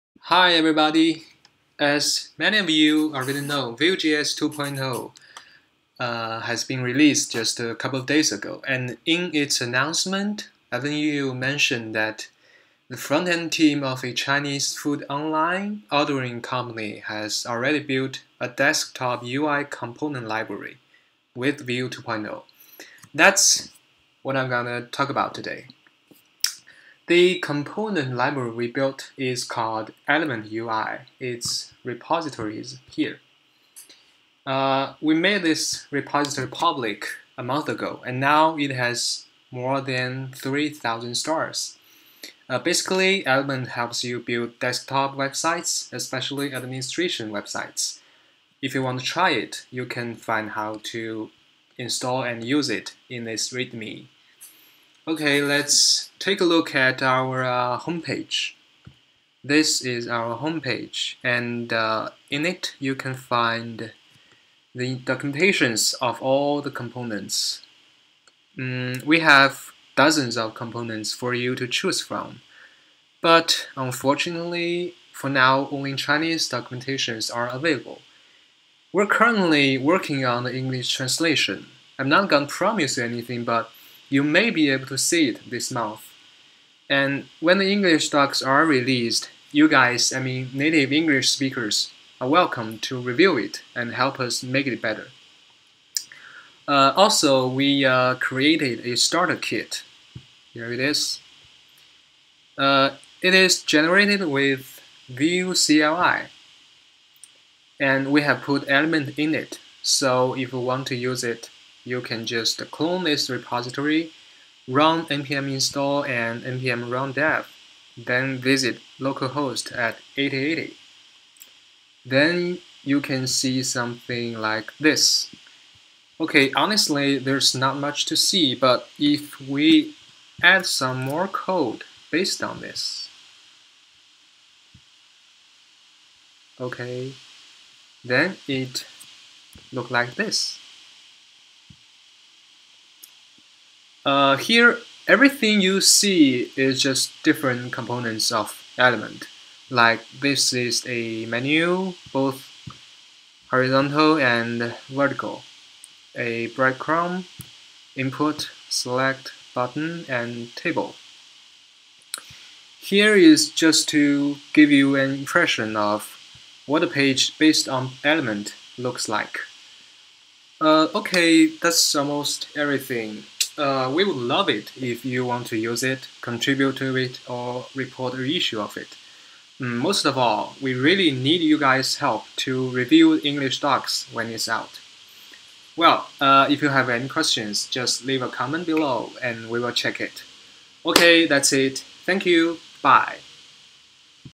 Element UI Introduction
Element UI is a desktop UI component library based on Vue 2.0